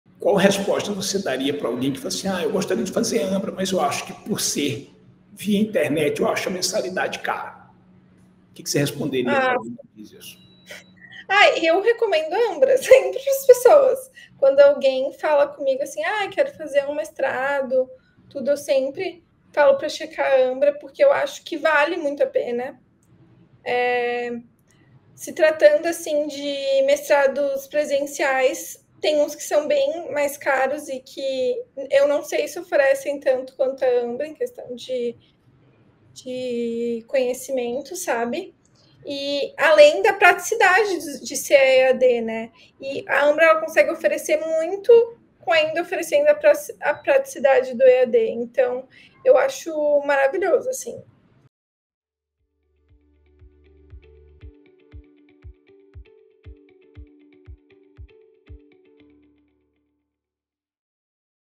Eu sempre recomendo a Ambra - Camila Balsini
Confira porque a advogada e mestranda Camila Balsini sempre recomenda a Ambra!